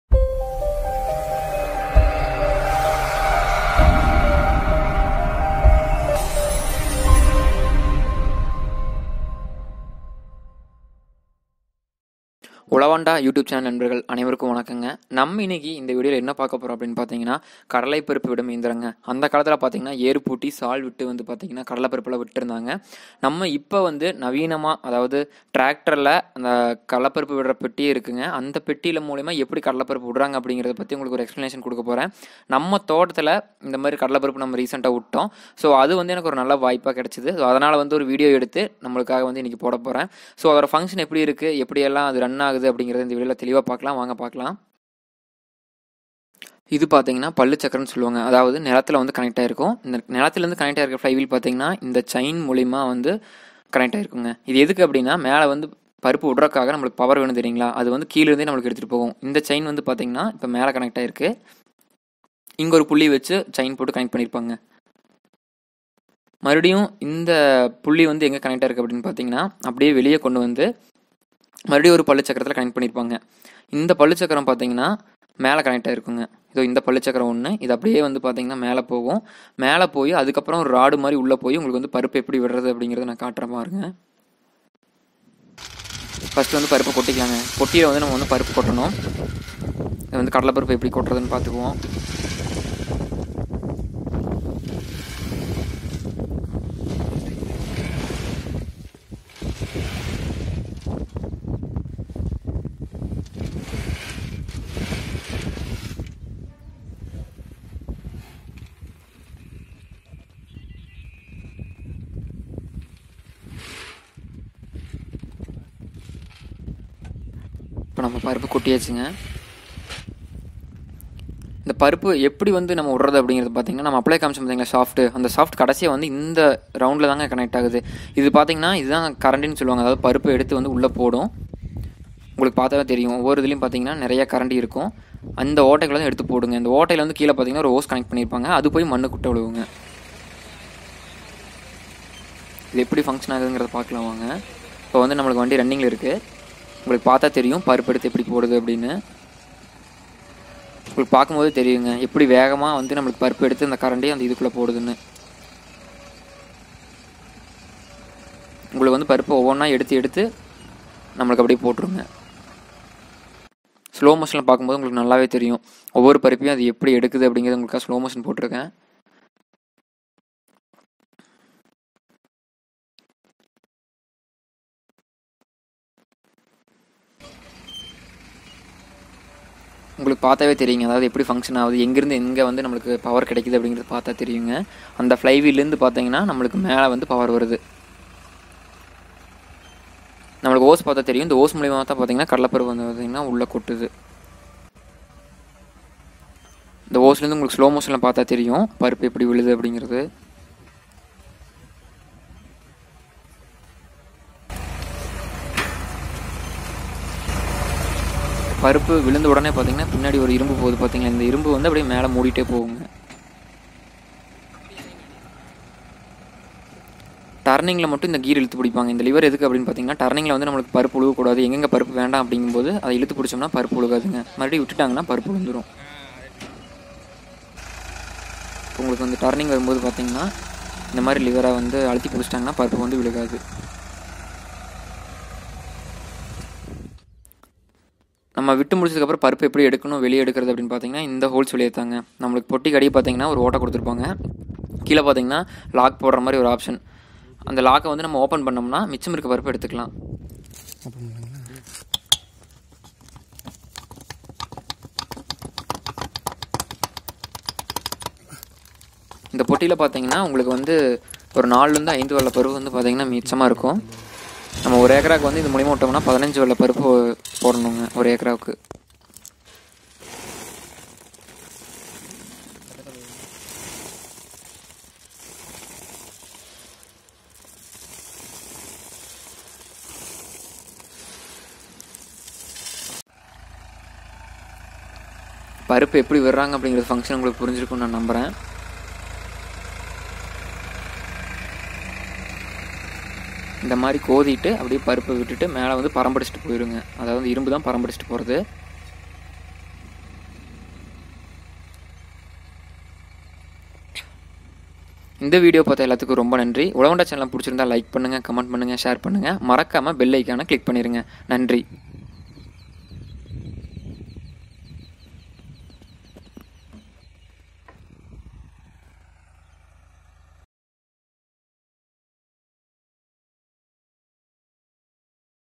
நிலக்கடலை விதைக்கும் இயந்திரம்! GROUNDNUT CULTIVATING MACHINE USING TRACTOR!! Quarantine work!!!
இந்த வீடியோ பதிவு முழுவதுமாக நிலக்கடலை விதைக்கும் இயந்திரத்தை பற்றியது டிராக்டர் பயன்படுத்தி எவ்வாறு நிலக்கடலை விதைக்கப்படுகிறது என்பதை இந்த வீடியோ தெளிவாக விளக்குகிறது !

This video is fully explaining about groundnut cultivating machine using tractor, in my land we are using tractors to cultivate groundnut , this is very interesting and knowledgeable!!!

எனது சொந்த நிலத்தில் டிராக்டர் பயன்படுத்தி நிலக்கடலை விதைக்கப்பட்டதை வீடியோவாக எடுத்துள்ளேன்!!!

இந்த வீடியோ அனைவருக்கும் ஷேர் பண்ணுங்க மறக்காம உழவன் டா சேனலை பண்ணுங்க!

என்றும் உங்கள் ஆதரவுடன்!!!